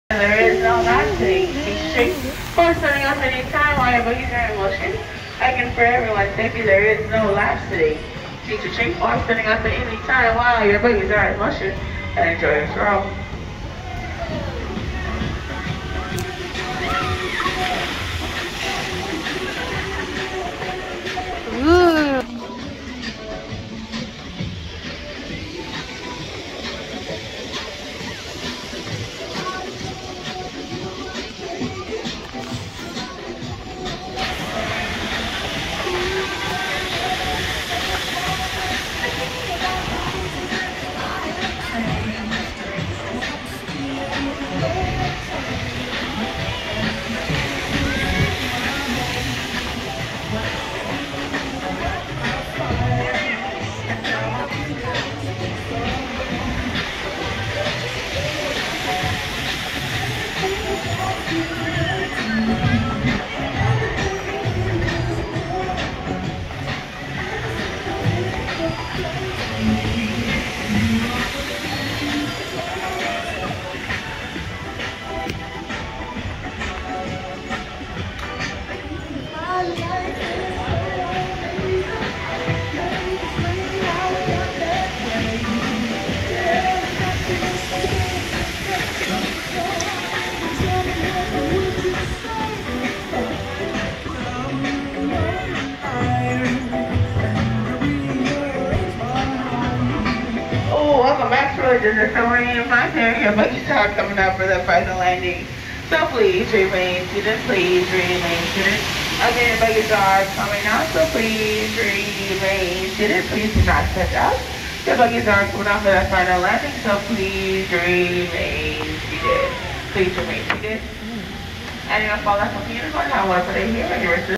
Super Silly Fun Land Super Silly Swirl POV Ride Through Universal Studios Hollywood
We spin around at sunset in Universal